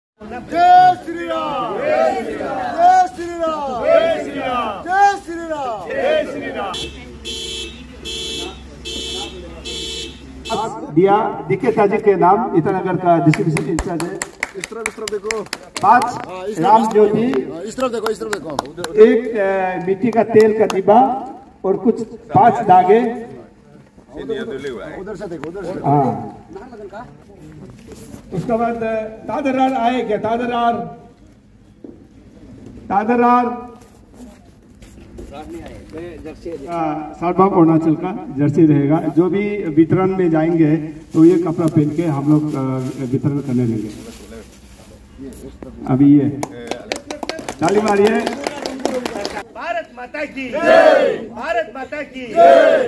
Jai Shri Ram Slogans Chanted By BJP Leaders While Distributing 10800 Diyas | Ram Mandir News
The inauguration of the Ram temple is a turning point in India's cultural history, culminating a journey spanning centuries that witnessed several legal and political events. The idol of Lord Ram was revealed during the 'Pran Pratishtha' ceremony, led by Prime Minister Narendra Modi.

Prime Minister Narendra Modi led the 'Pran Pratishtha' rituals in Ayodhya's Ram Mandir on Monday as the temple was inaugurated, marking the homecoming of Lord Ram after 500 years. The first picture of Ram Lalla (child Lord Ram), sculpted by Mysuru-based sculptor Arun Yogiraj, was revealed to the world after the scarf covering the idol's eyes was taken off